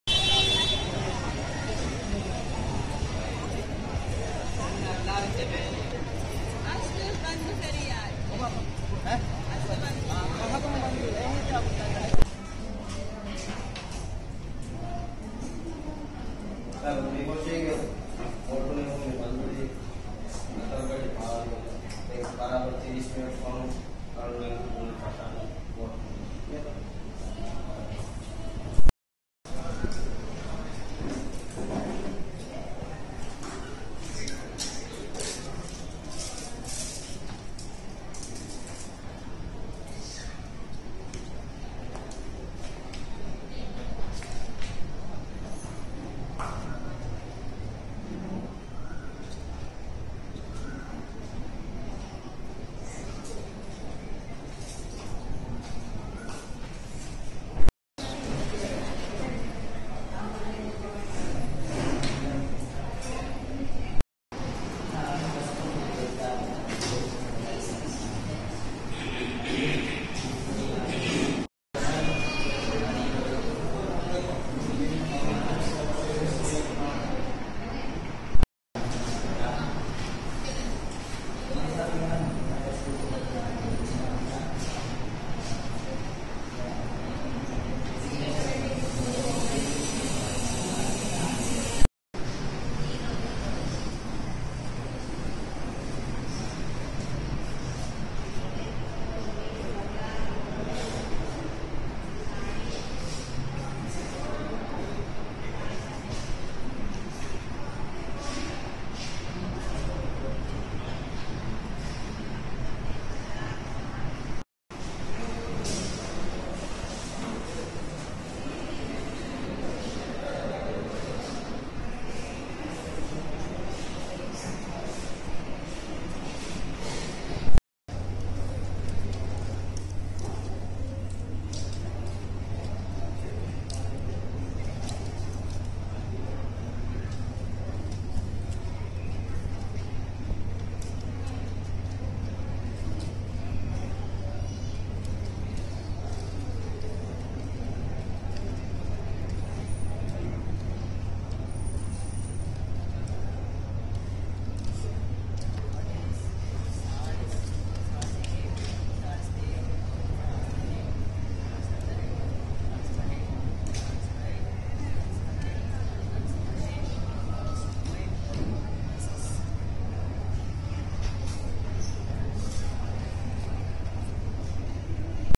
Counting of ballots in parliamentary polls
Officials of the Elections Commission count ballots cast during the parliamentary elections on March 22, 2014. Haveeru video: Mohamed Sajid